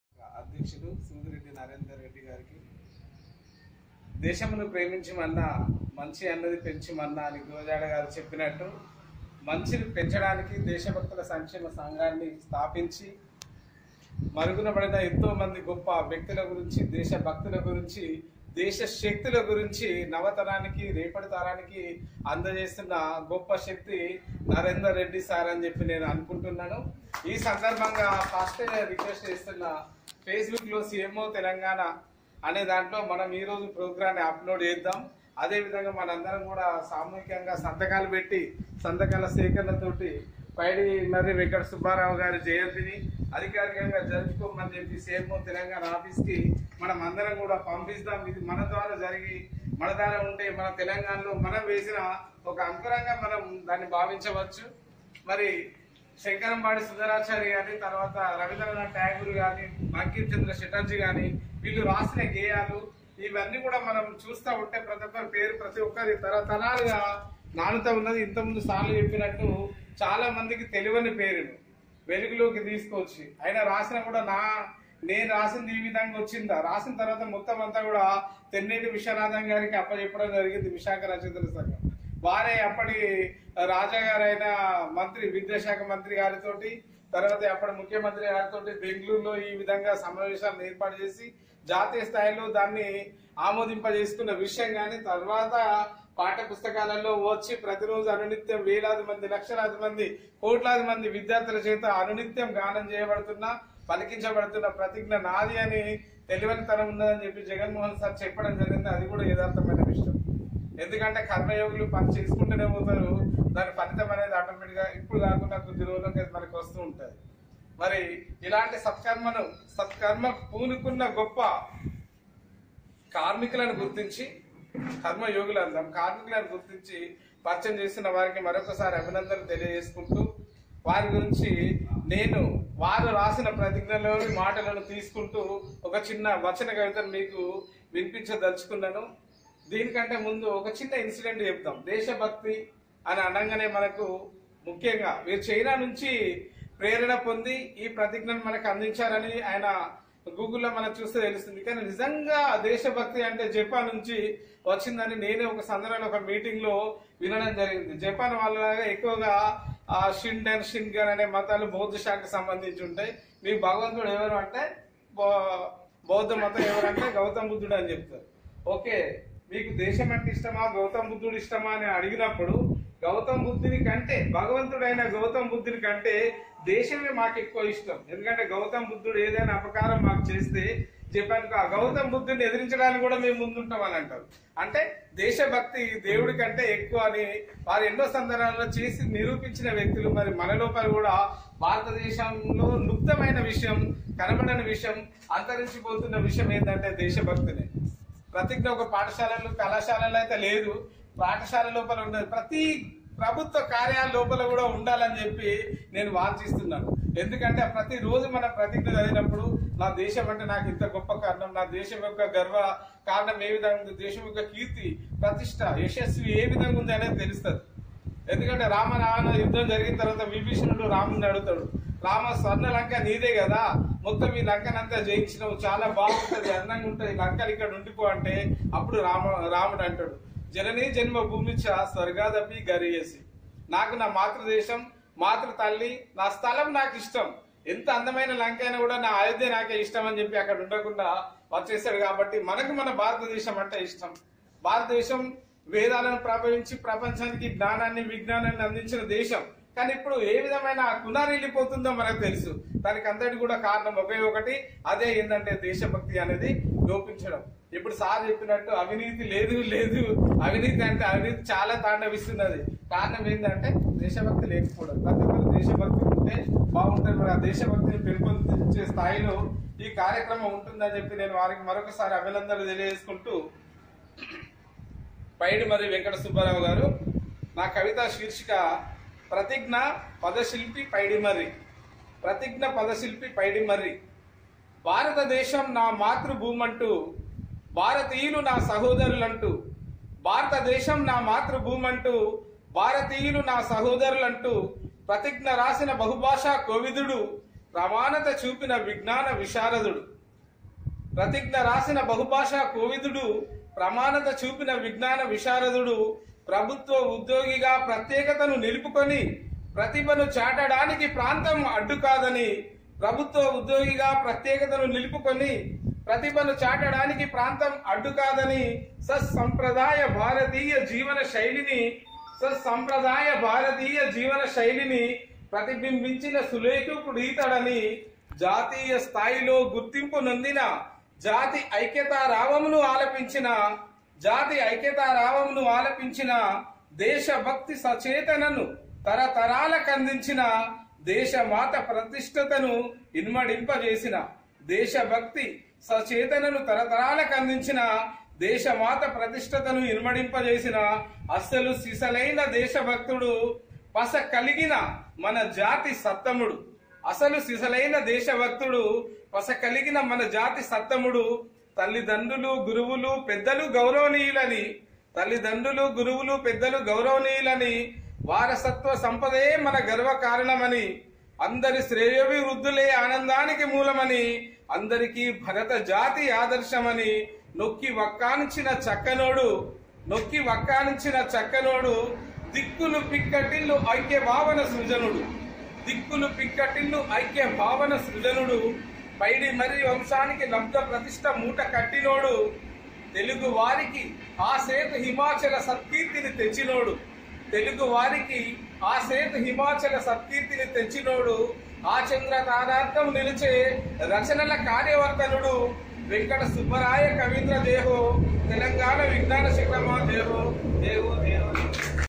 ప్రతిఙ్ఞ రూపశిల్పి పైడిమర్రి
ప్రతిఙ్ఞ పదశిల్పి పైడిమర్రి
  భారతదేశం నా మాతృభూమంటూ
  భారతీయులు నా సహోదరులంటూ
  ప్రతిఙ్ఞ రాసిన బహుభాషా కోవిధుడు
  ప్రమాణత చూపిన విఙ్ఞాన విశారదుడు
  ప్రభుత్వ ఉద్యోగిగా ప్రత్యేకతను నిలుపుకొని
  ప్రతిభను చాటడానికి ప్రాంతం అడ్డుకాదని
  సత్సంప్రదాయ  భారతీయ జీవనశైలిని
  ప్రతిబింబించిన సులేఖకుడీతడని
  జాతీయ స్థాయిలో గుర్తింపు నొందిన
  జాతి ఐక్యతారావమును ఆలపించిన
  దేశభక్తి సచేతనను తరతరాలకందించిన
  దేశమాత ప్రతిష్టతను ఇనుమడింప జేసిన
  అసలు సిసలైన  దేశభక్తుడు
  పస కలిగిన మనజాతి  సత్తముడు
  తల్లిదండ్రులు గురువులు పెద్దలు గౌరవనీయులని
   వారసత్వ సంపదయే మన గర్వకారణమని
   అందరి శ్రేయోభివృద్దులే ఆనందానికి మూలమని
   అందరికి భరతజాతి  ఆదర్శమని
   నొక్కి వక్కానించిన చక్కనోడు
   దిక్కులు పిక్కటిల్లు ఐక్యభావన సృజనుడు
   పైడిమర్రి వంశానికి లబ్ద ప్రతిష్ఠ  మూటకట్టినోడు
   తెలుగు వారికి ఆసేతు హిమాచల సత్కీర్తిని
    తెచ్చినోడు
    ఆ చంద్రతారార్కము నిలచే రచనల      కార్యవర్తనుడు
    వెంకట సుబ్బరాయ కవీంద్ర జయహో !!
    తెలంగాణ విఙ్ఞాన శిఖరమా జయహో!!